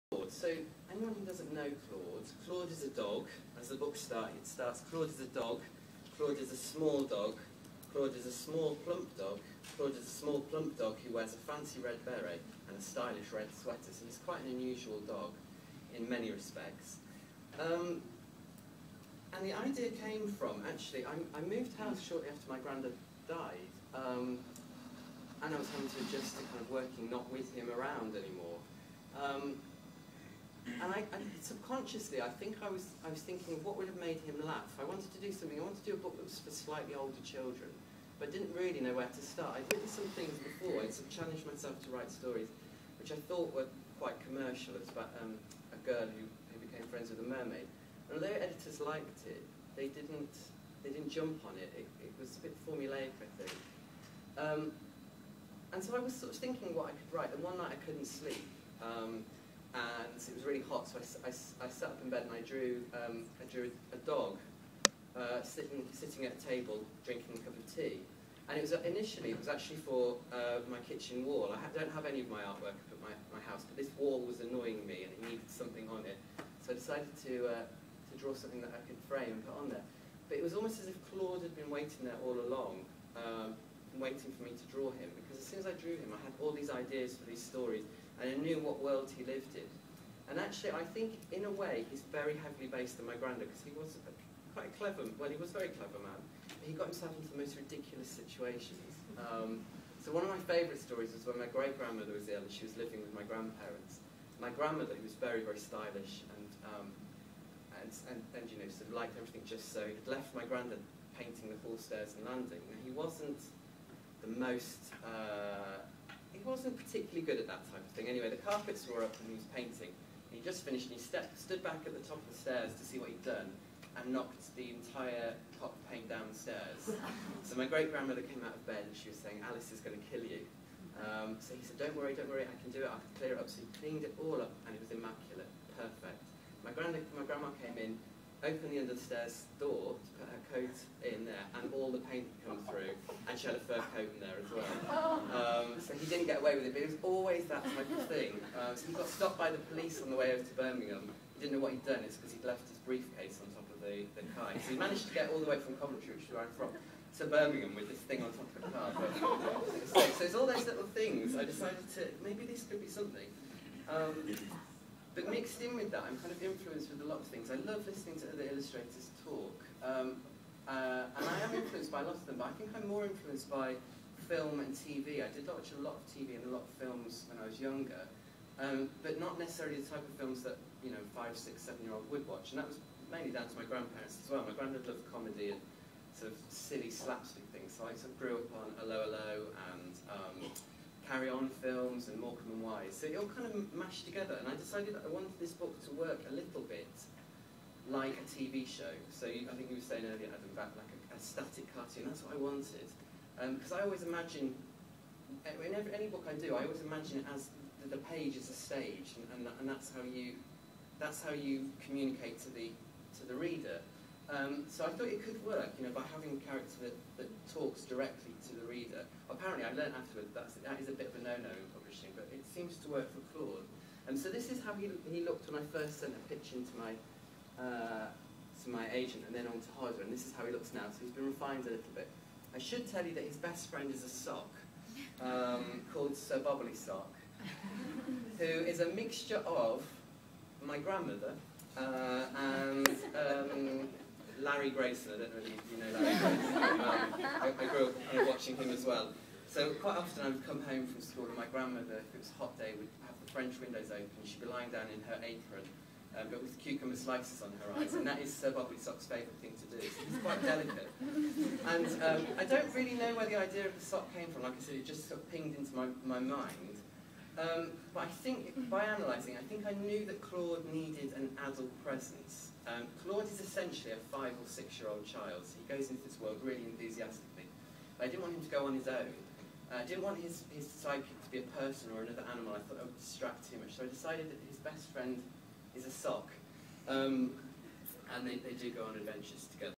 Alex T Smith talks about Claude
Author and illustrator Alex T Smith talks about how the character Claude evolved into a series for younger readers. He was speaking to an audience of training teachers at Kingston University.